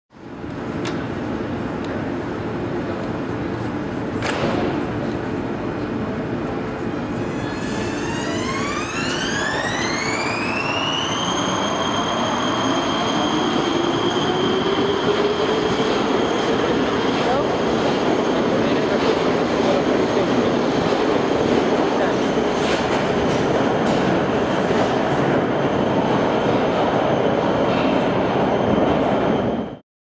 Trasa C, souprava Siemens M1, odjezd na obratiště na Letňanech
Dopravní podnik hlavního města Prahy a.s.